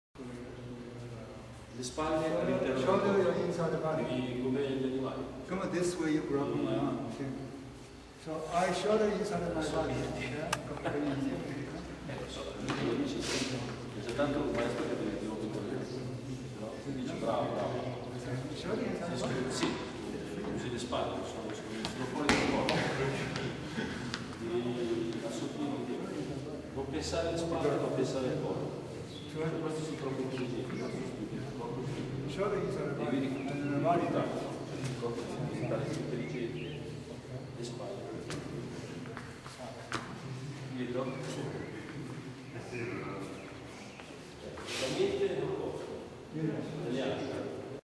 structure power
in martial arts shoulder must be inside the body